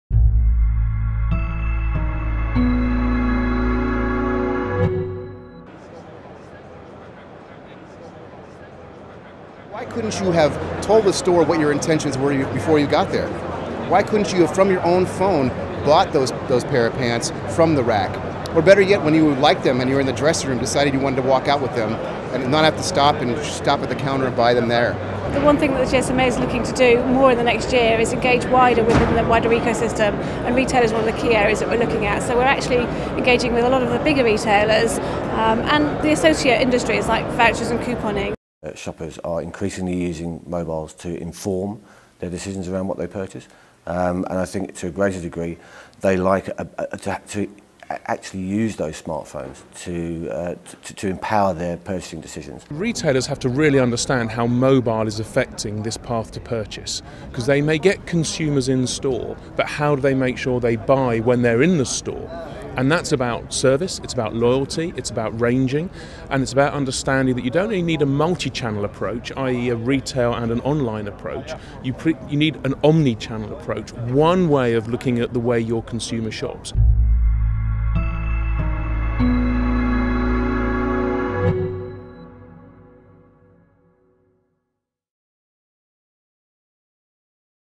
Omni-Channel Retailing
With shoppers increasingly reliant upon their mobiles for purchasing and information, retailers are adopting an omni-channel strategy. To watch the full-length video, download the Sync Magazine app from the Apple AppStore